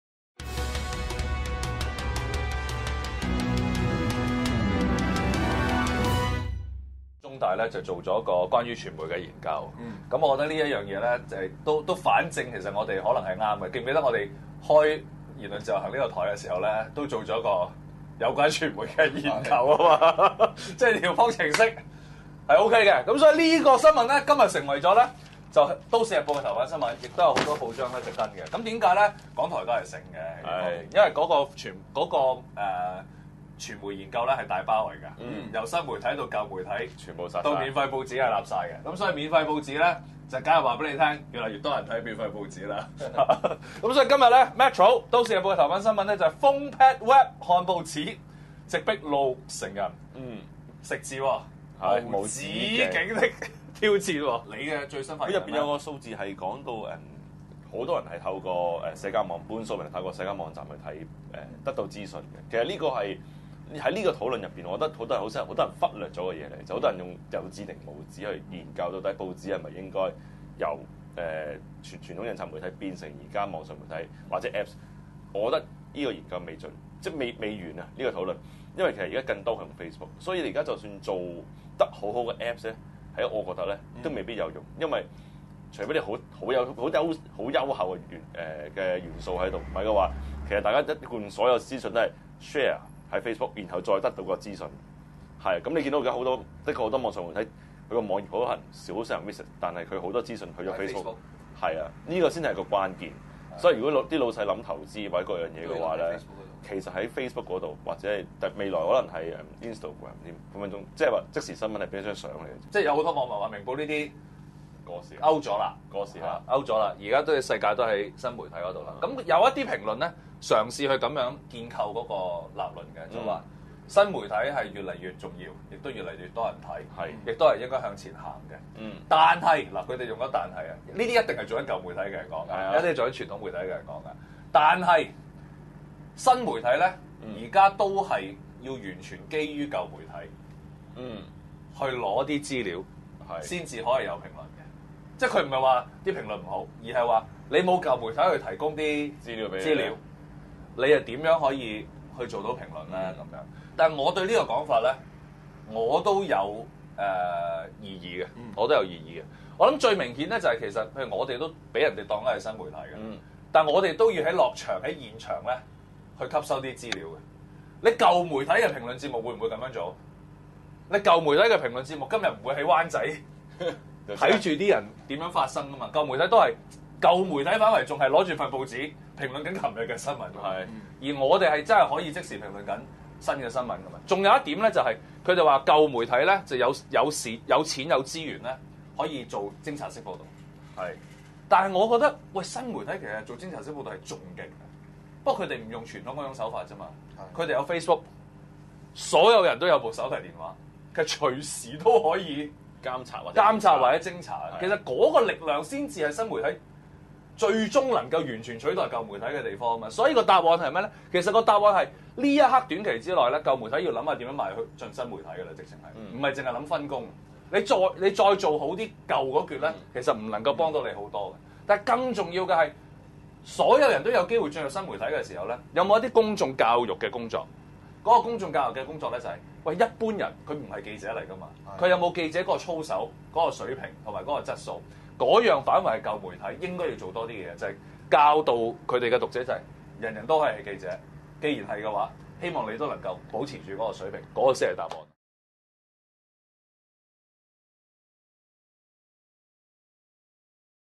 世界終究是屬於新媒體的？
2014 年 1 月 14 日
A1解困：新媒體年代，更需薪火相傳

用 apps 或上網閱報已是趨勢，比率跟閱讀印刷報紙一樣，各佔六成，按此趨勢，可見新媒體將越來越壯大。舊傳媒人覺得，新媒體的內容以至評論多來自傳統媒體，但想深一層，新媒體能做到評論當日新聞，舊媒體的評論，仍是慢了一天。今時今日，人人一手機，偵查報道可以隨時做，新媒體終將能取代舊媒體的原因，莫過於此？

主持：黃永　張鼎源　林子傑
地點：灣仔分域街